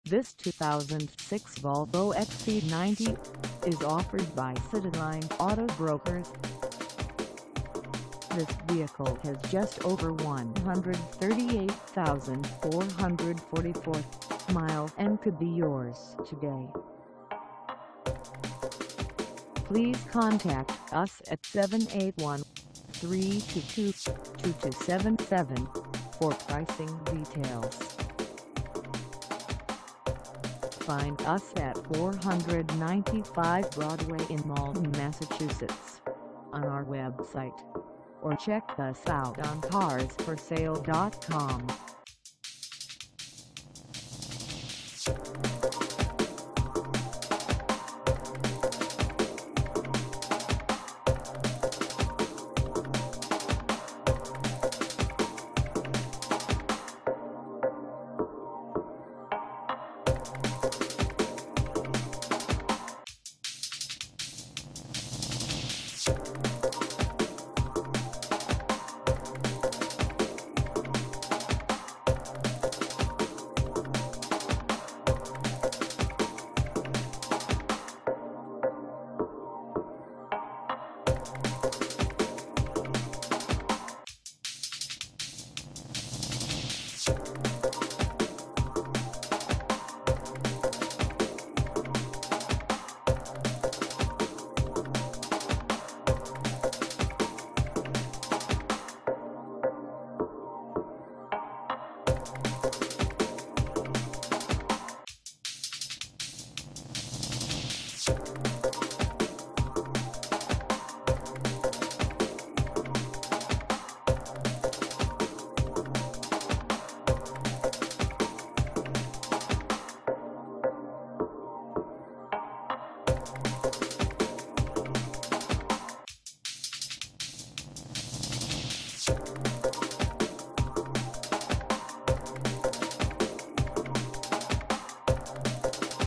2006 Volvo XC90 for sale in Malden, MA 02148 at the Citylin
This 2006 Volvo XC90  is for sale in Malden, MA 02148 at Cityline Auto Brokers.
Engine: I5 turbo 2.5
Transmission: Automatic
Color: Green
Mileage: 138444
Contact Cityline Auto Brokers 495 Broadway  Malden, MA 02148 to test drive this 2006 Volvo XC90  today.